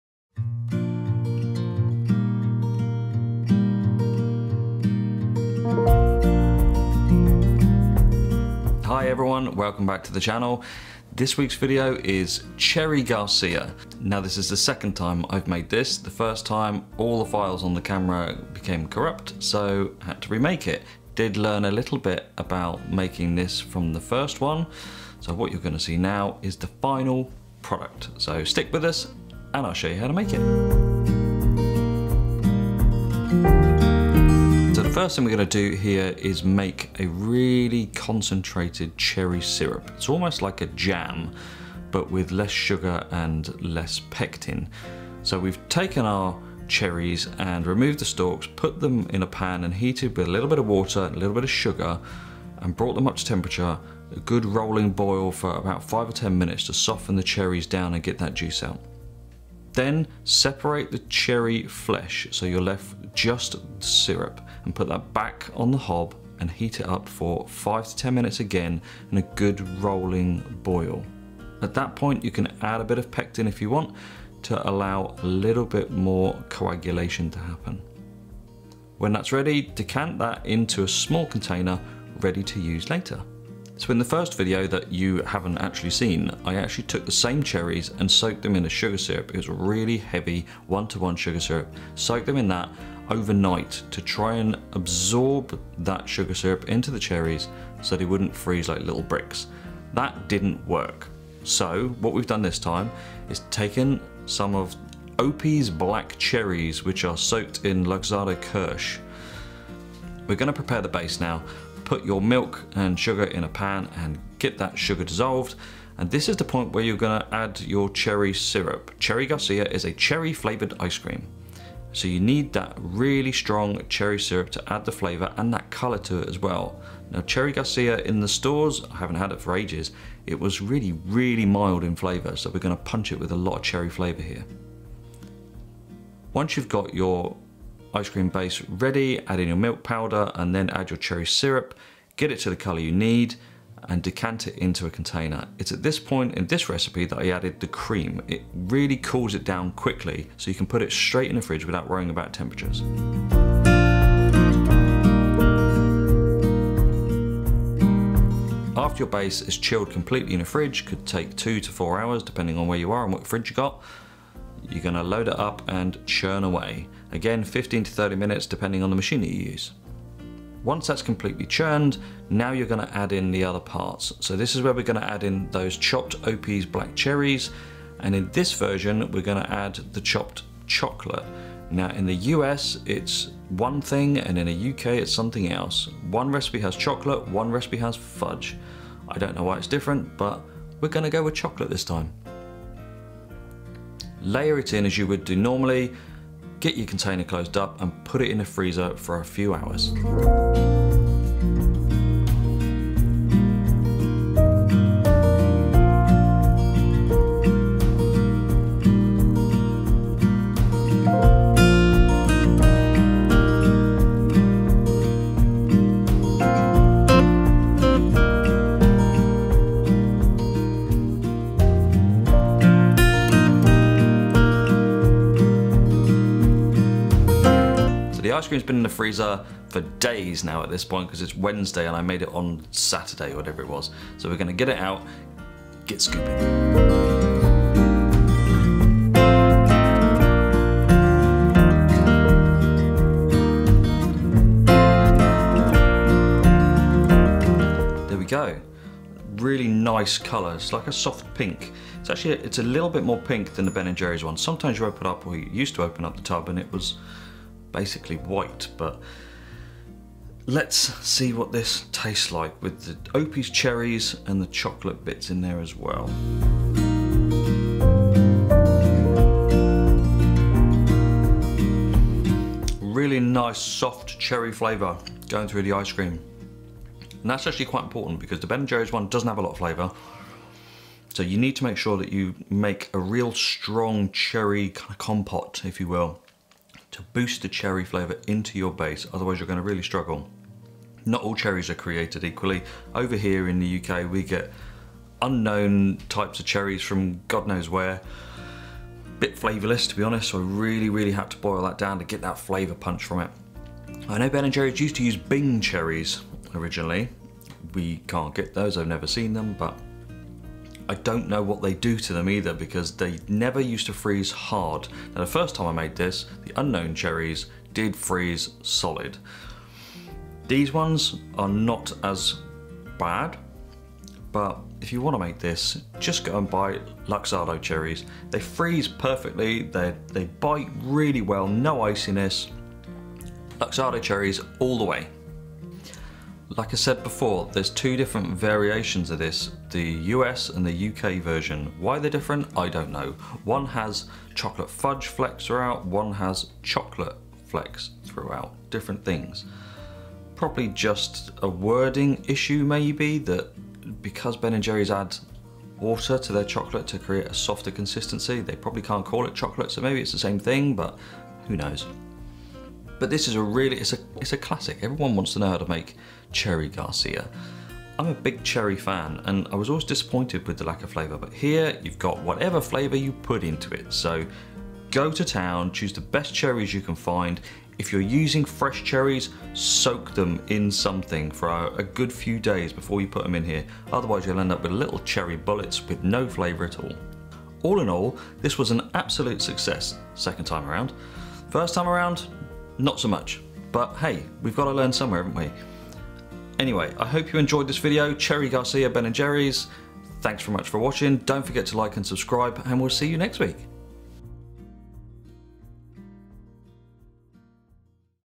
Ben & Jerry's Cherry Garcia - Ice Cream Perfection
After the first version of this went a bit sideways (thanks Fuji), I went about making this again.
A fan favourite named after a guitarist, it's no longer available in the UK but is in the US (sorry, I don't know about other locations).

It's a Cherry Ice Cream with Cherry pieces and either chocolate chunks (uk) or fudge flecks (US) depending on where you are.

What you'll need:

240ml cream (50% fat)
300ml milk (1.8% fat)
80g sugar
60g Cherry concentrate
2tsp Cherry liqueur if wanted
2tbsp Skimmed Milk Powder
50g chopped (70% chocolate)
50g maraschino cherries chopped (use Luxardo but Ben & Jerry's use Bing Cherries)

The type of cherries have a big impact on the flavour so you can change the base just by changing the cherry.